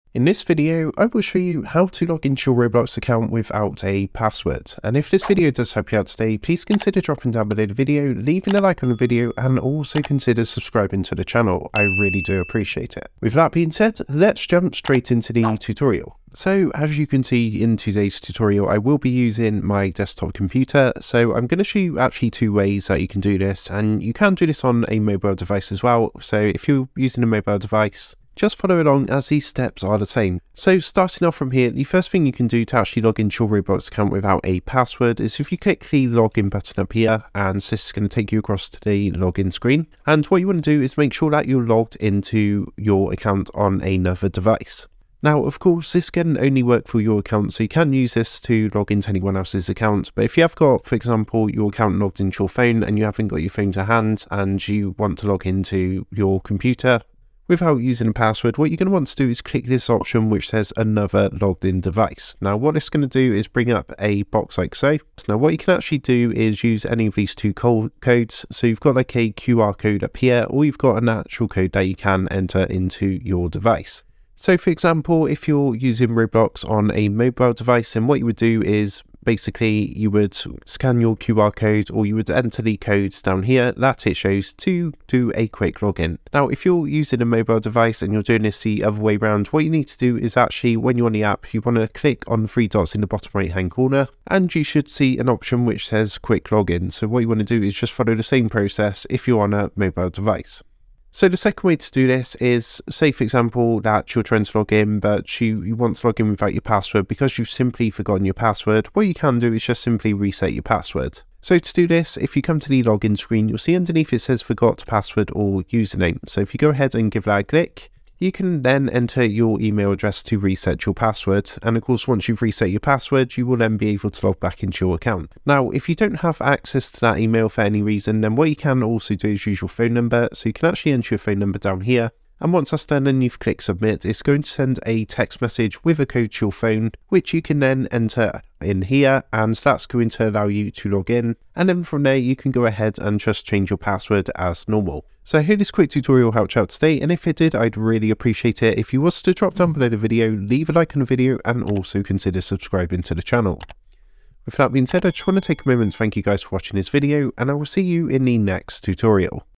How To Login To Your Roblox Account Without Password - Step By Step Guide
How To Login To Your Roblox Account Without Password

In this video, we'll show you how to login to your Roblox account without a password. Whether you've forgotten your password or simply want a more convenient way to access your account, this tutorial is for you. We'll walk you through the process of setting up alternative login methods, such as using your email or phone number, and guide you on how to recover your account in case you're locked out. By the end of this video, you'll have all the knowledge you need to easily access your Roblox account without a password, ensuring a seamless gaming experience.

 ✨Recommended VPN: ✨

Thanks!